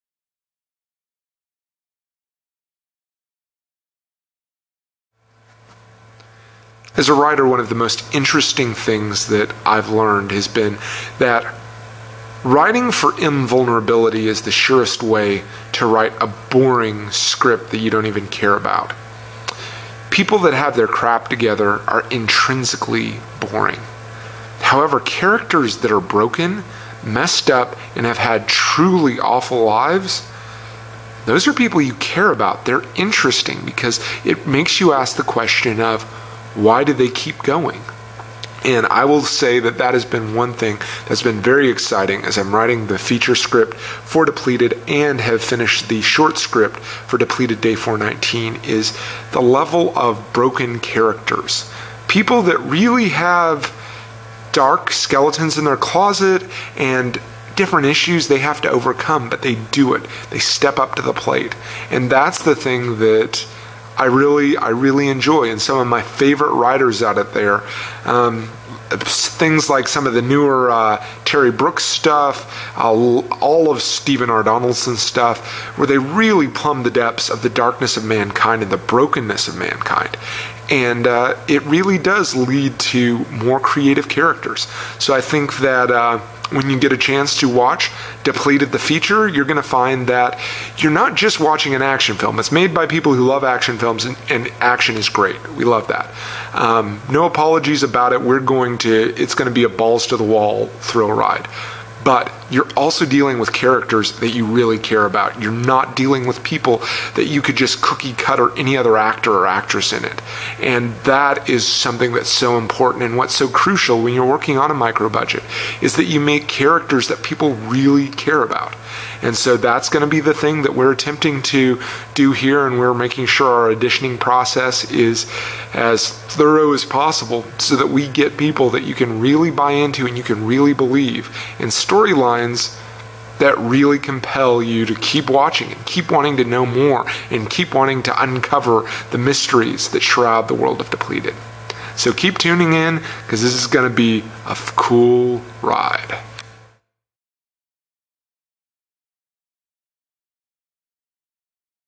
Depleted Writers Vlog #1: Why Broken Characters Are The Only One's Worth Exploring
Screenwriter Jeremy Hanke discusses why the characters in Depleted are who they are and how their human blend of brokenness and courage makes them worth exploring and getting to know in the midst of an action film.  He talks a bit about his literary influences, like Stephen R. Donaldson.